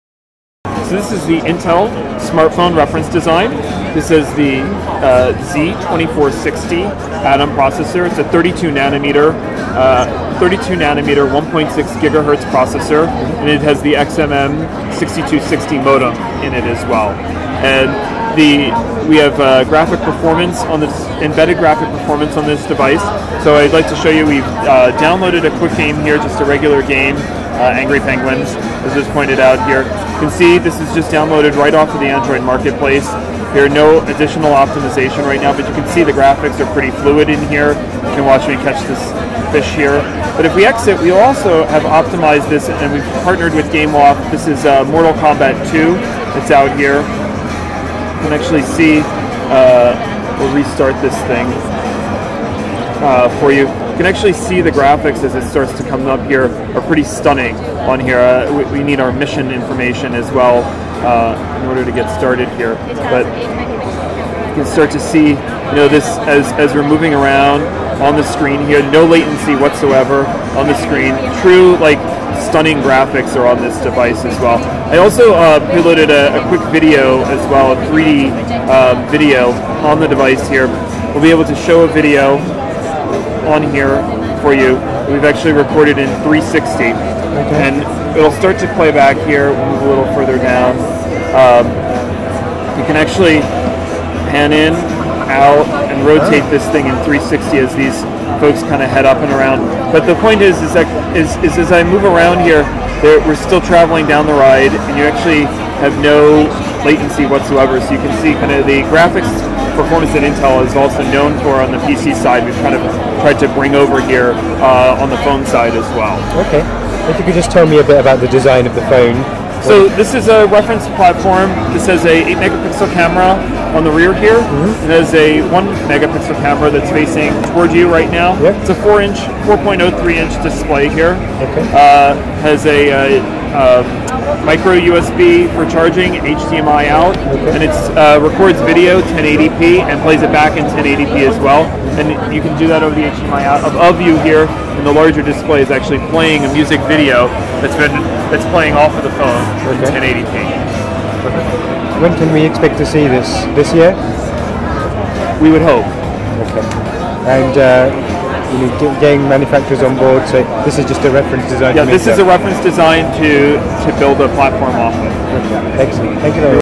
Intel Atom prototype smartphone video demo
Intel is gearing up to push into the smartphone market by launching Android devices that run Intel Atom Medfield chips. The chip maker has created a reference design to give manufacturers an idea of what the final design could look like.

Key specifications include the 1.6GHz Intel Atom Medfield single core processor, a 4.03in screen, 8-megapixel camera and support for Android Ice Cream Sandwich. There is no set release date yet, but Intel will need to move as quickly as possible if it wants to take on ARM.